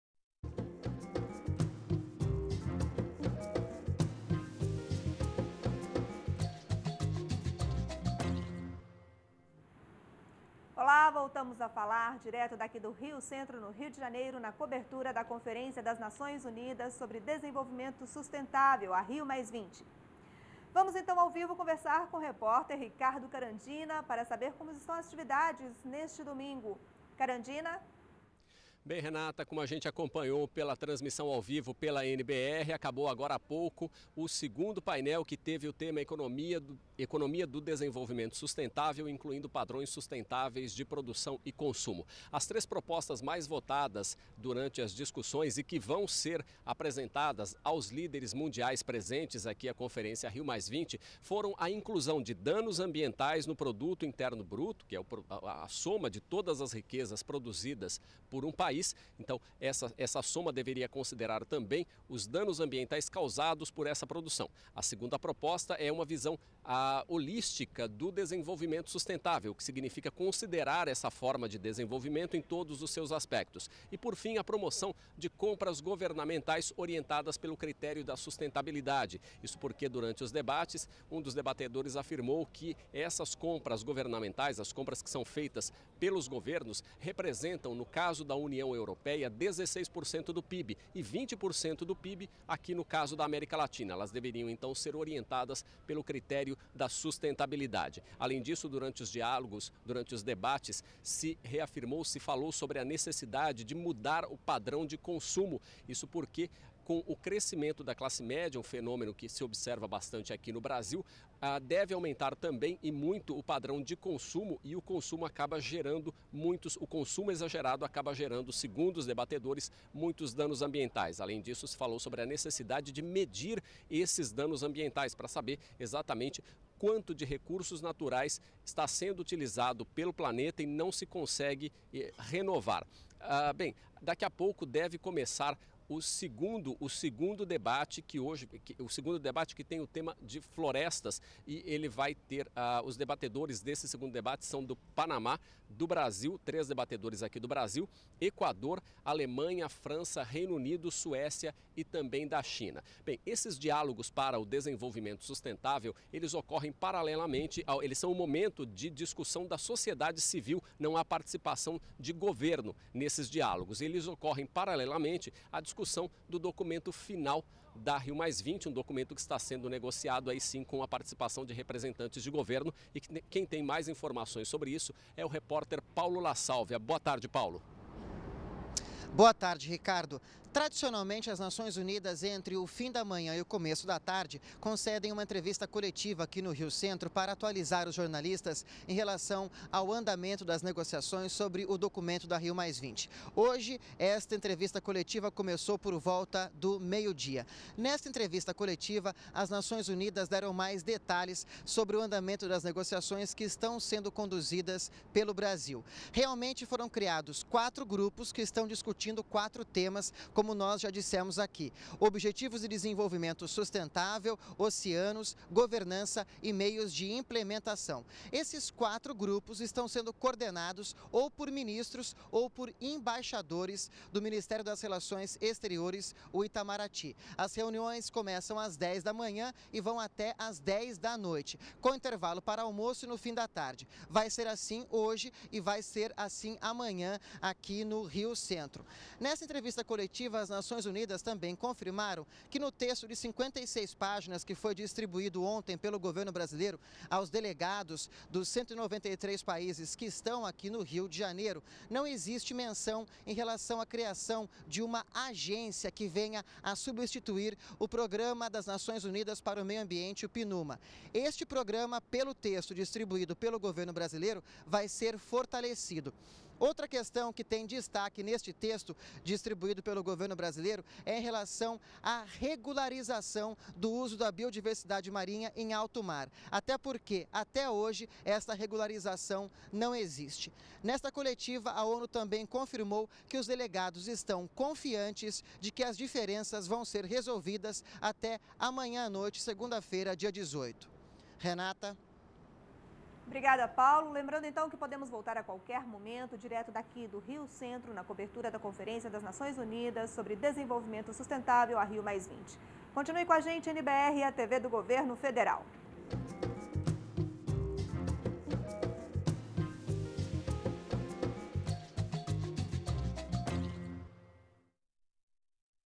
Mundanças no padrão de consumo foi assunto de painel na Rio+20
ESPECIAL RIO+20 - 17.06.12: Na cobertura da Rio+20, os repórteres Ricardo Carandina e Paulo La Salvia trazem informações sobre as discussões durante os painéis "Diálogos para o Desenvolvimento Sustentável", que estão acontecendo na Conferência. Além disso, necessidade de mudança nos padrões de consumo foi assunto discutido em um dos painéis deste domingo (17). O texto do documento final continua em discussão na Conferência.